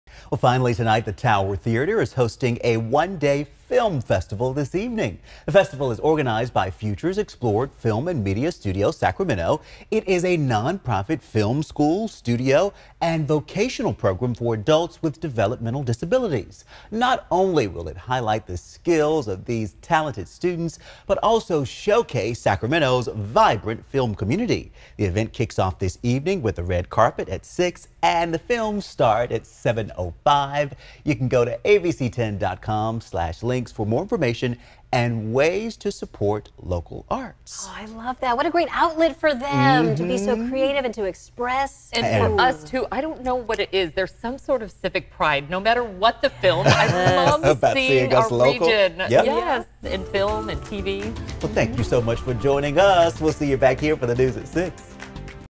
Tower Theater hosts 'Future Explored' film students festival
Futures Explored Film and Media Studio Sacramento will be holding a one-day film festival at Tower Theatre in Sacramento on Wednesday, November 16.